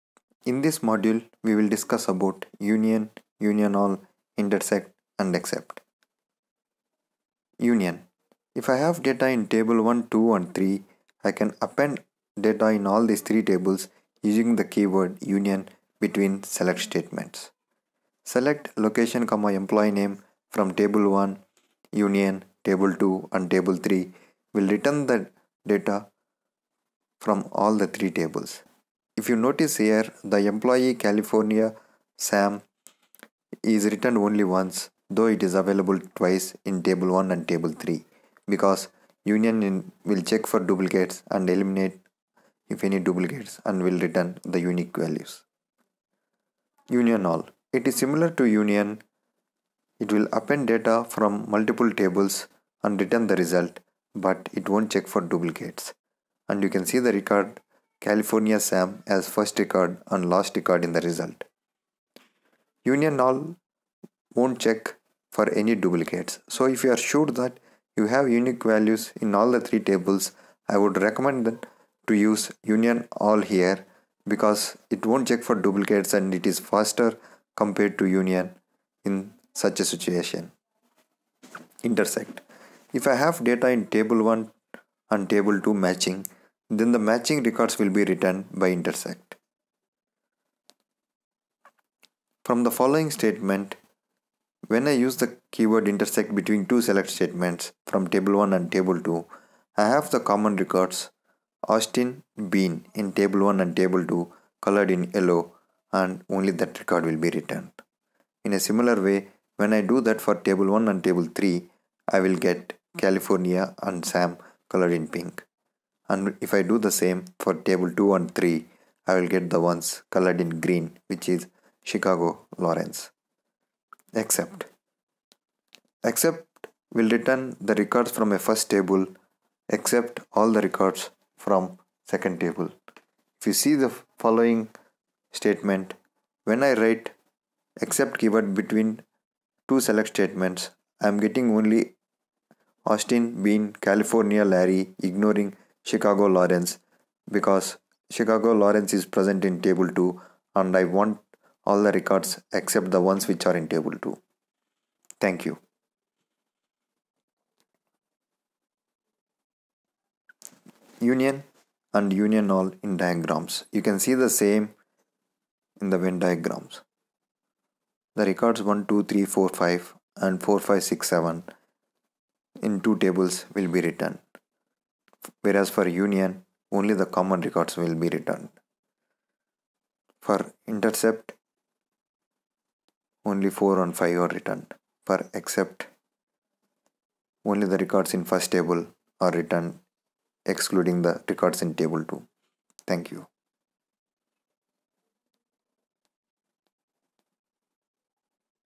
#12 Database Union, Union all, Intersect and Except
This is a full course on AWS Redshift. We cover the Architecture, Schema and Table creation with SORT and DIST Styles, SQL functions, Performance tuning, WLM creation, etc., Please subscribe to this channel for more interesting videos and drop a comment for any AWS videos on demand.